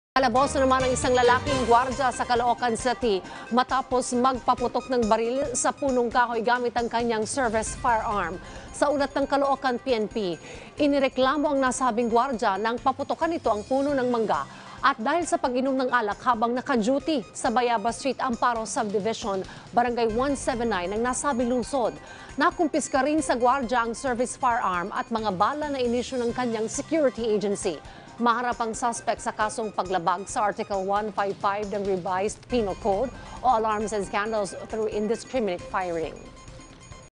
Guard sa Caloocan, arestado matapos magpaputok ng baril sa puno gamit ang kanyang service firearm
Guwardiya sa Caloocan, arestado matapos magpaputok ng baril sa puno gamit ang kanyang service firearm